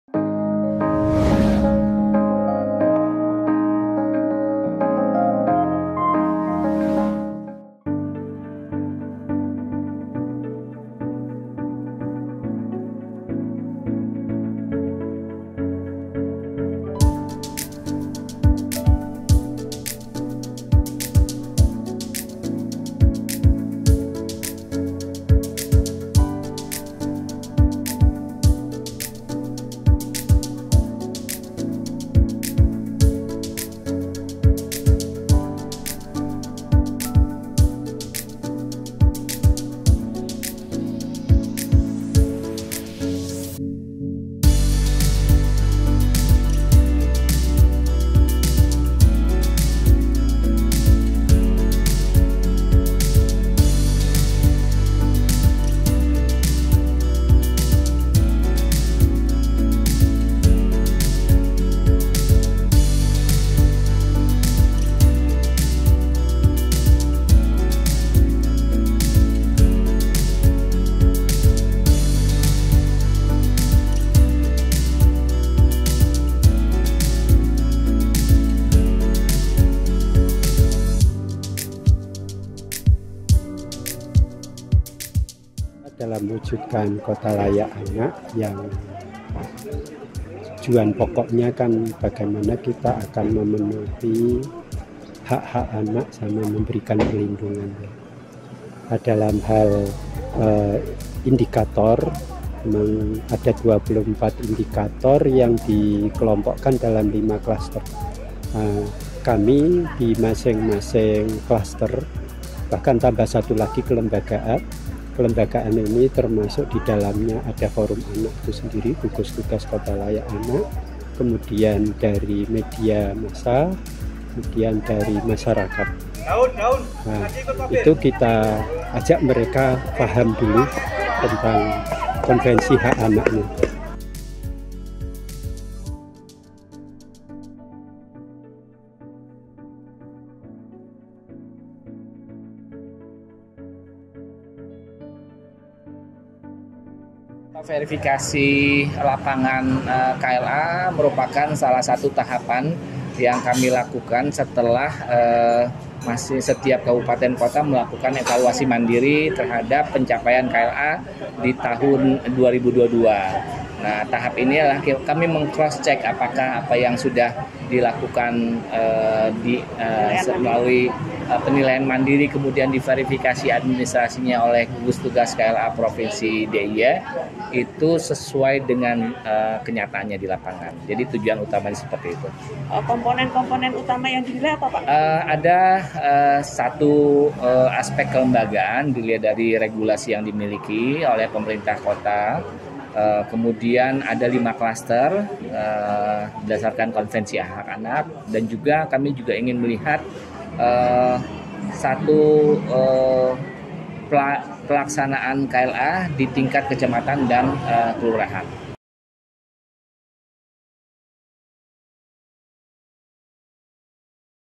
Komitmen Pemkot Jogja: Melindungi dan Memenuhi Hak Anak - Warta YK
Temukan bagaimana Pemerintah Kota Yogyakarta, dengan tekad yang kuat, melindungi dan memenuhi hak-hak anak. Penghargaan Kota Layak Anak yang mereka raih selama enam tahun berturut-turut adalah bukti nyata komitmen mereka. Tetapi, lebih dari sekadar gelar, Pemkot Yogyakarta fokus pada memberikan pelayanan terbaik dan kebaikan bagi anak-anak kita. Bergabunglah dalam perjalanan ini dan saksikan upaya mereka dalam melindungi masa depan yang cerah!

Dalam verifikasi lapangan terbaru, tim dari Kementerian Pemberdayaan Perempuan dan Perlindungan Anak mengevaluasi berbagai aspek perlindungan anak di kota ini. Mulai dari kelembagaan hingga pelayanan ramah anak, Pemkot Yogyakarta berkomitmen untuk memberikan yang terbaik bagi generasi masa depan. Bergabunglah dalam perjalanan mereka dan saksikan bagaimana mereka menjaga dan memperjuangkan hak-hak anak di Kota Yogyakarta yang kita cintai.

Kontributor peliput dan videografer: Tri D & Dhimaz Janoe